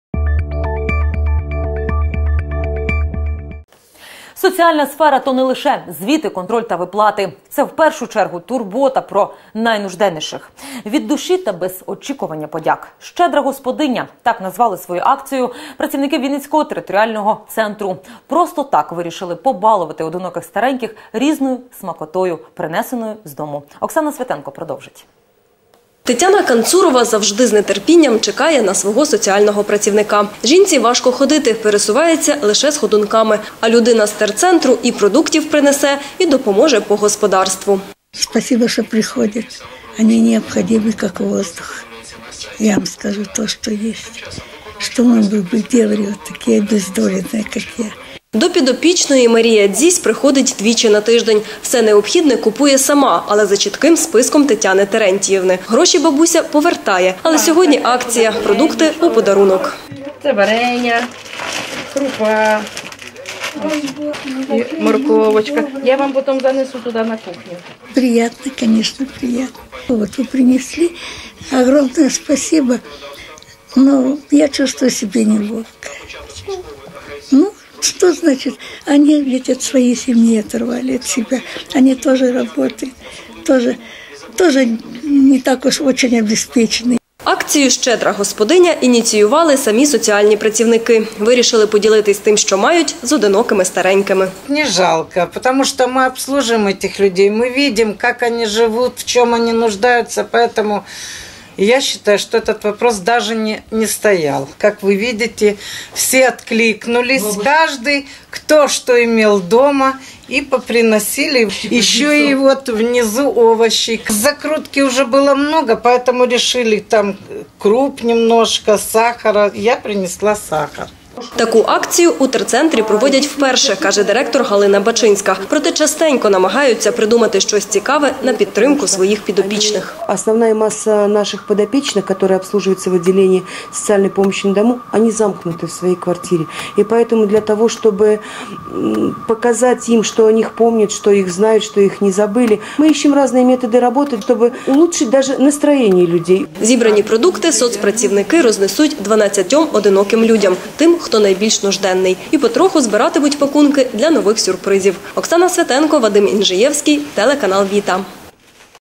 Телеканал ВІТА Новини 2014-03-21 Щедра господиня
«Щедра господиня» - акцію з такою назвою влаштували для своїх підопічних соціальні працівники вінницького територіального центру. Вони принесли з дому хто що міг: закрутки, овочі, крупу, цукор. Продукти розносять найбільш нужденним.  Таких на обліку терцентру -- 12.